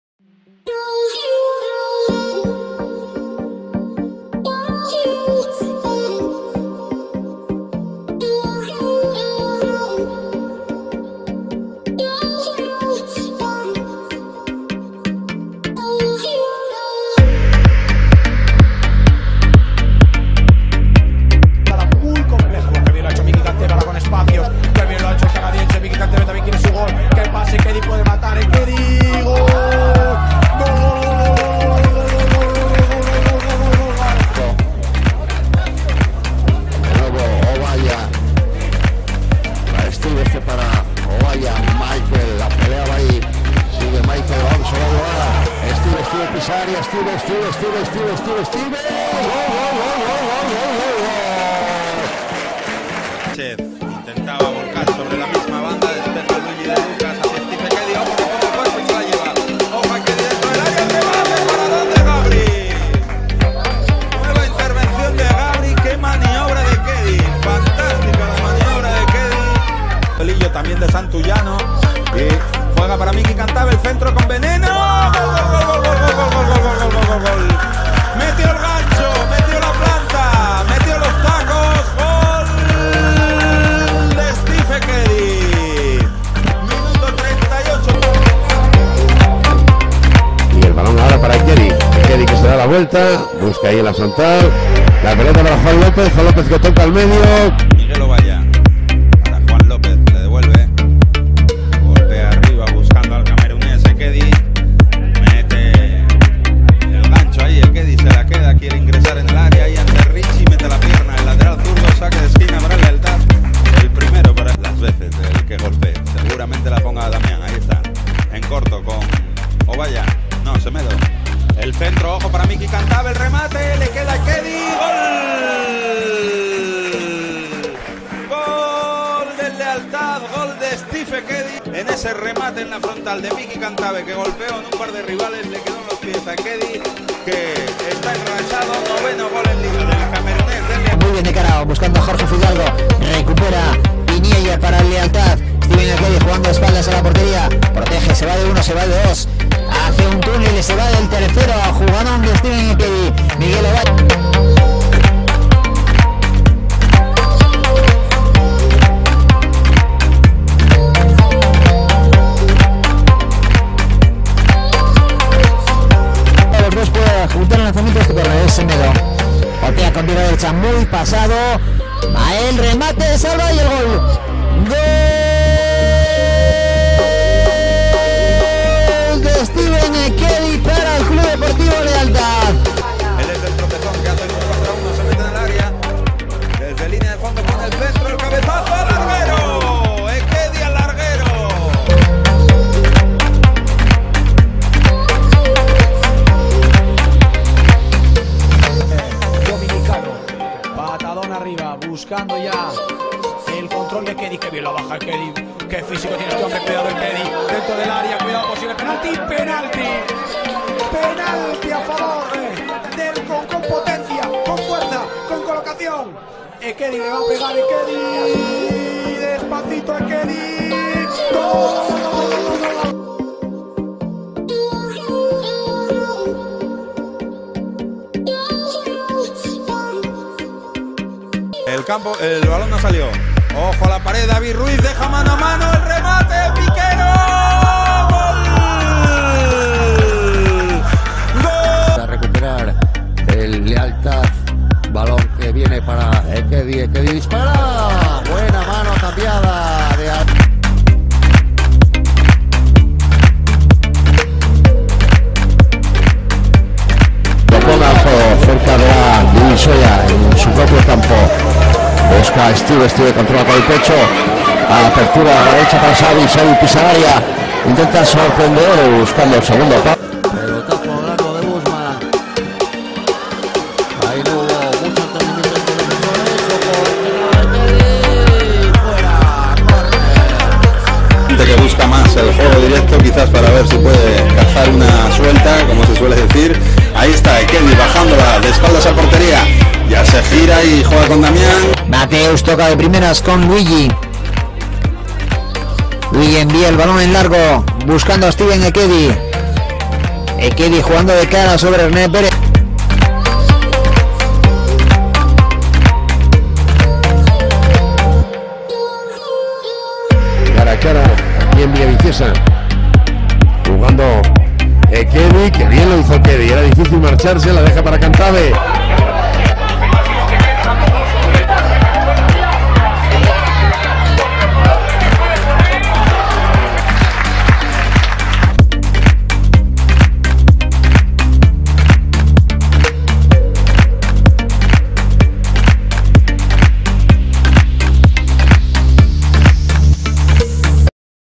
Steve EKEDI - CD LEALTAD Highlights 2019-2020 Vol. 1
STEVE EKEDI Central Forward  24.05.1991
Nationality : Cameroon         190CM   85KG
CD LEALTAD ( Spain Tercera Division ) Season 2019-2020